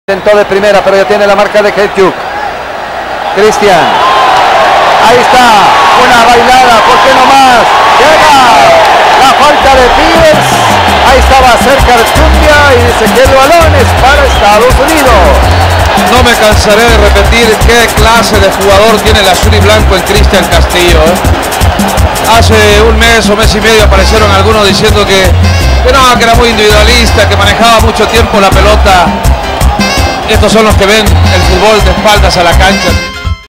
Christian Castillo [Olé!] vs. USA 2009
La magia/gran jugada de Cristian Castillo vs. Frankie Hedjuk en un partido entre El Salvador vs. USA/Estados Unidos en 2009. Eliminatorias Rumbo a Sudafrica 2010 desde el Estadio Cuscatlan.